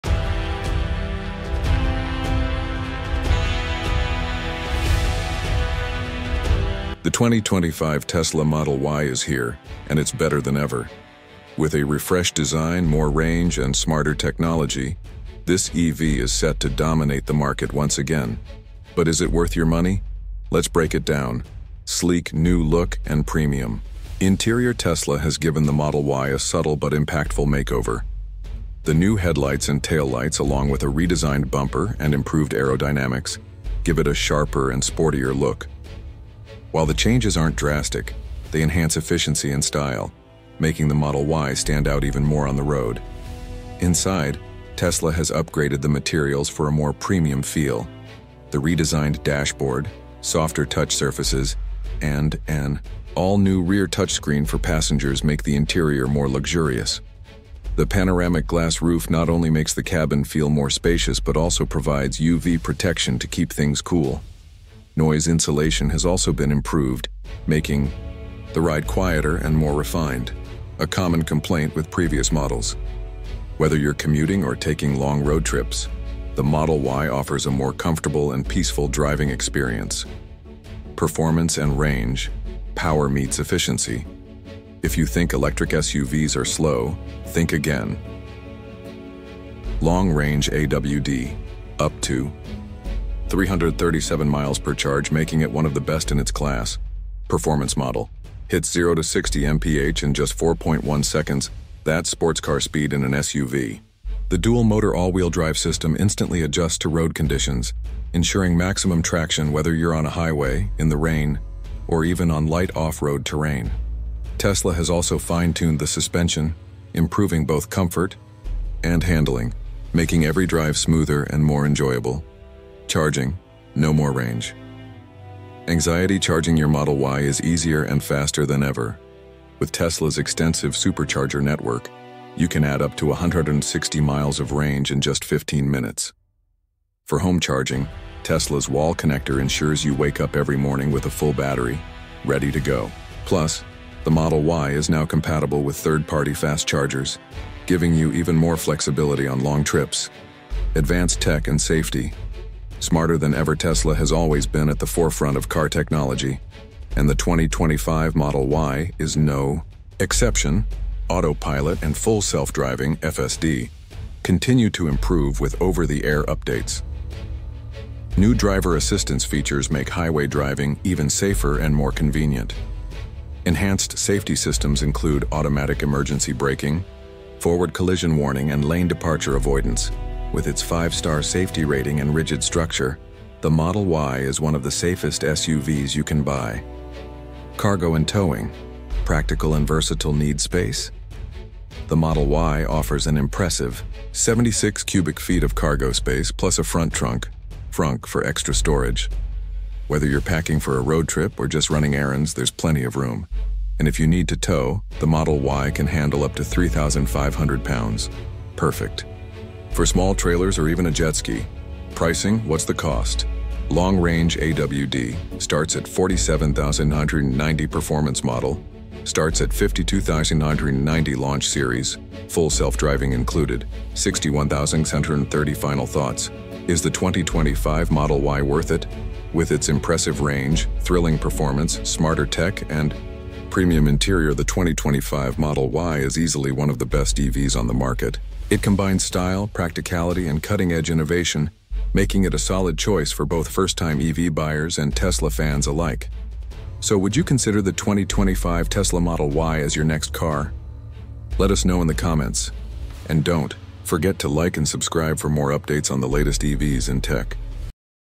2025 Tesla Model Y Review The Ultimate Electric SUV with Impressive Range & Performance!
The 2025 Tesla Model Y has arrived, and it's better than ever! From a sleek new design and premium interior to incredible performance and advanced tech, this electric SUV is setting new standards. In this video, we break down everything you need to know about the Model Y, including range, charging, safety features, and pricing. Is this the electric vehicle you’ve been waiting for? Watch now to find out!